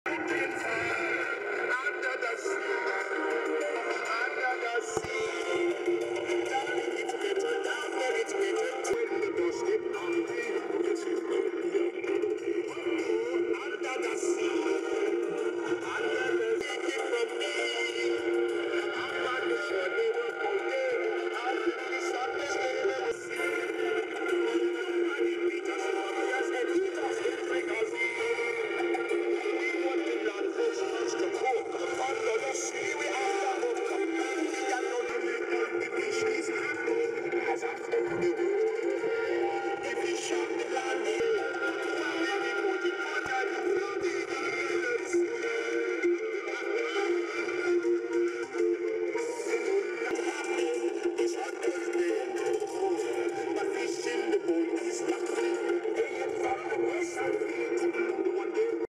Raw: Divers, Snorkelers at Undersea 'Concert'
Several hundred divers and snorkelers have submerged in the Florida Keys National Marine Sanctuary for a "concert" beneath the sea broadcast by a local radio station. (July 12)